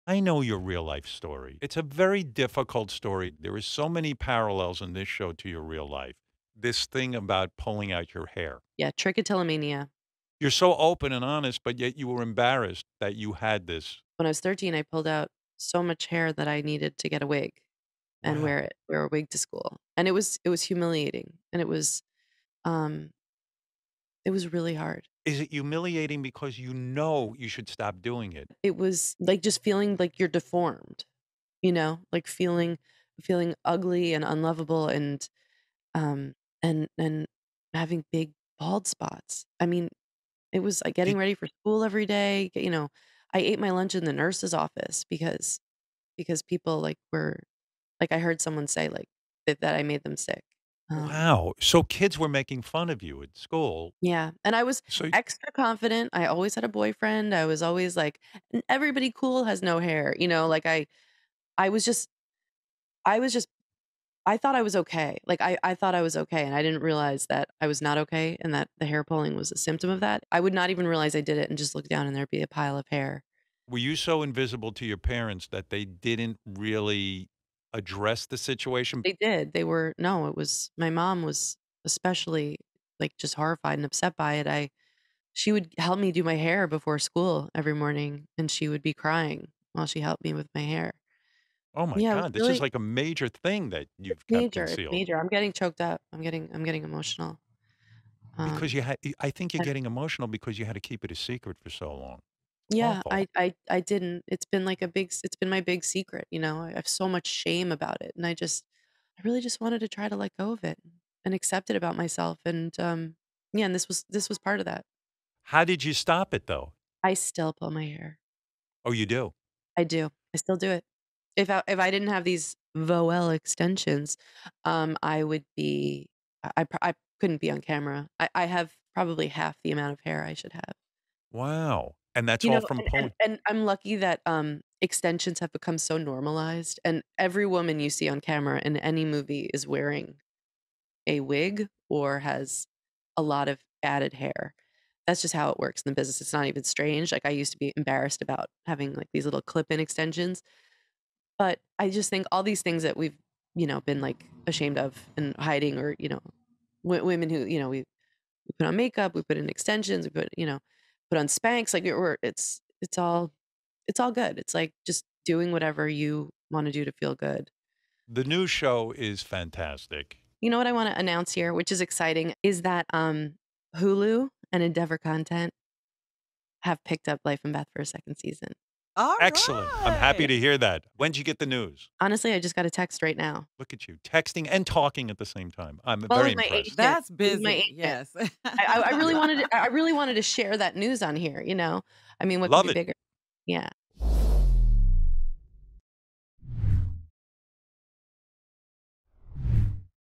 Amy Schumer Opens Up About Having a Hair-Pulling Disorder
Amy Schumer opens up about having trichotillomania and explains why she wanted to write the hair-pulling disorder into her Hulu show, “Life & Beth.”